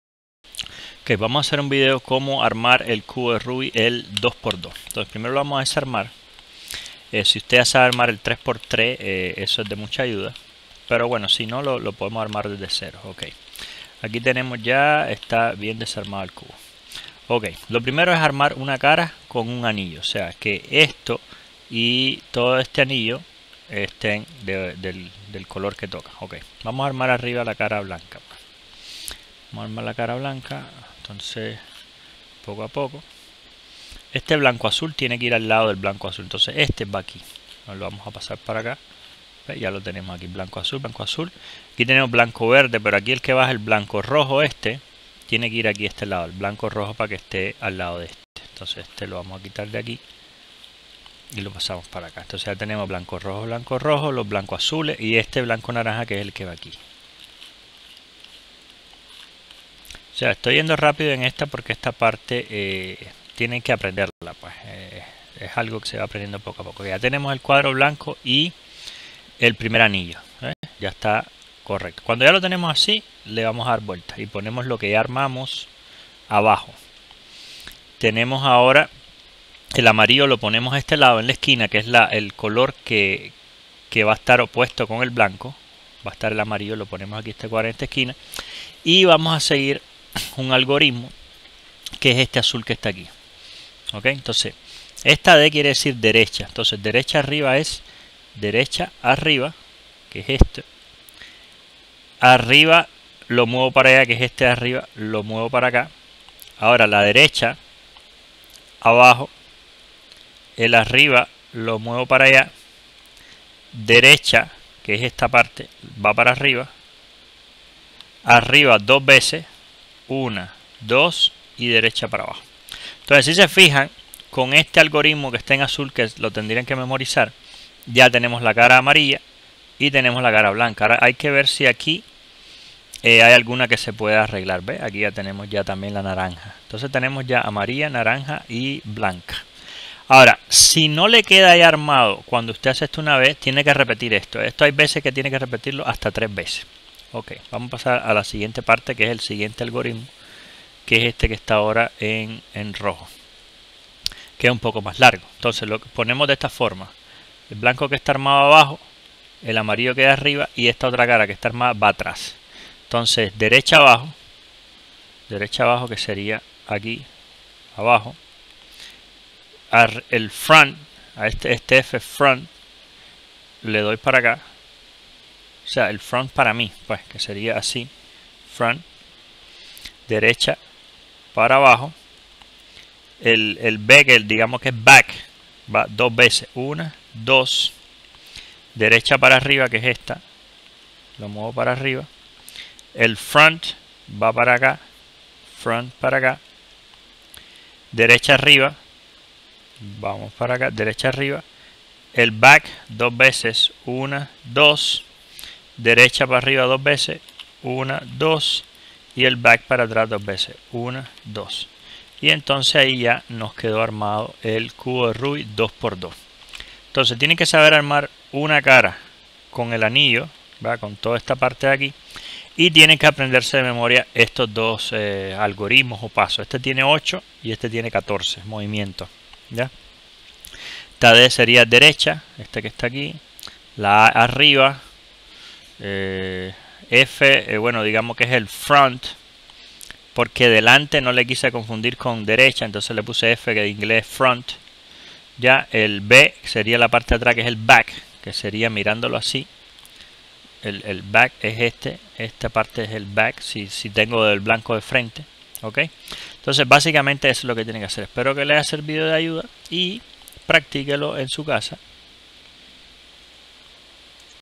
Como armar el Cubo de Rubik 2x2
Facil video para armar el Cubo de Rubik, interesante aprender el 2x2 si ya sabes el 3x3.

Aca pueden comprar el cubo 2x2